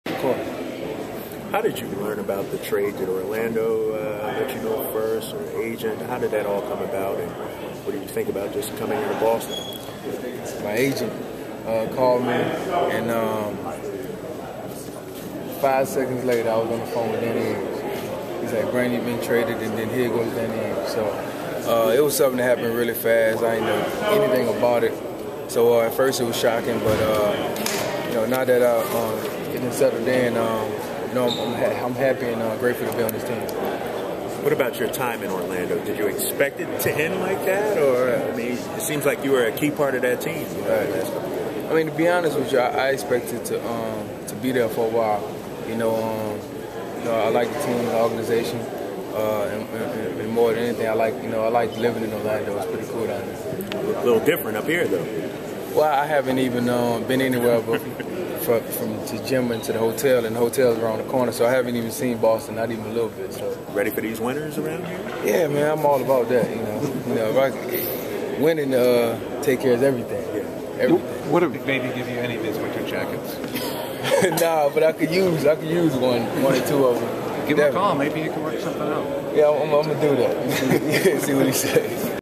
Brandon Bass is happy to be with the Celtics.flv
WALTHAM, Mass. -- Brandon Bass said he was happy to trade places with "Big Baby" Glen Davis after a trade last week sent him from the Orlando Magic to the Boston Celtics.